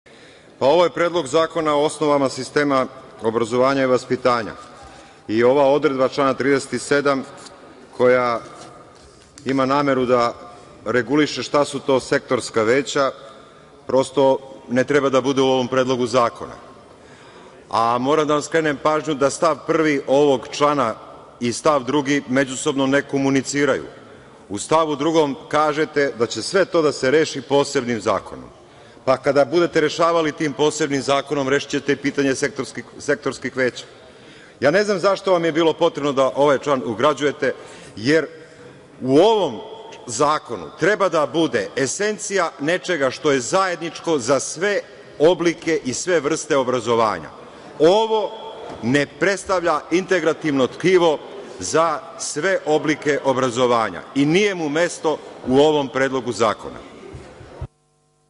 Zoran Krasić: Zašto ste ovaj član ugradili u ovom Predlogu zakona? Ovde mu nije mesto!
Rasprava u pojedinostima o Predlogu zakona o osnovama sistema o obrazovanju i vaspitanju i Predloga zakona o visokom obrazovanju.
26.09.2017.